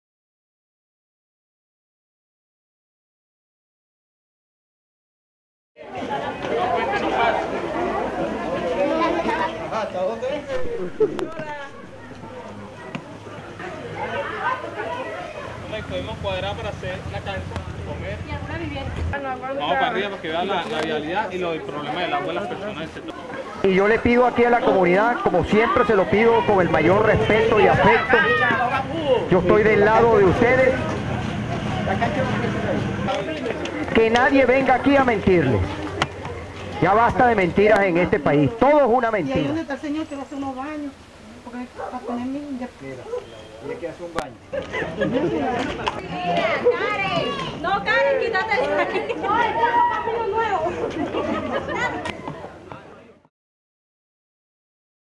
Conoce como son los recorridos del Gobernador Capriles (Cogollal, Guarenas)
Este es el trabajo que día a día hace el Gobernador Henrique Capriles. Todos los días en la calle, en una comunidad distinta, escuchando y solucionando los problemas junto a los mirandinos. Sobran razones para unirnos!